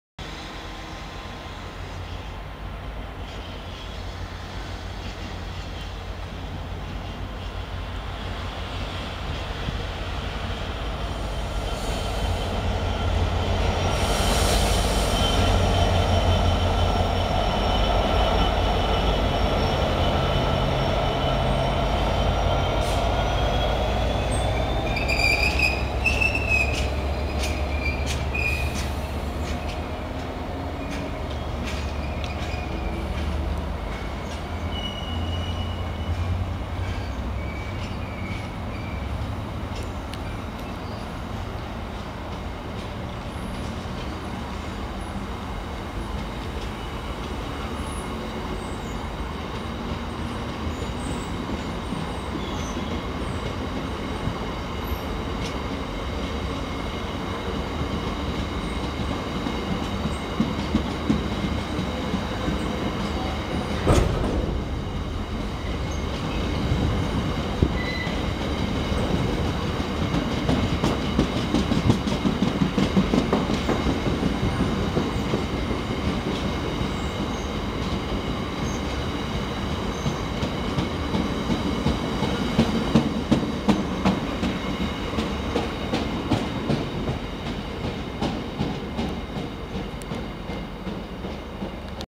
66303 at Leicester
Filmed 04/11/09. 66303 heads through Leicester working 4Z21 1000 Ratcliffe-Daw Mill Colliery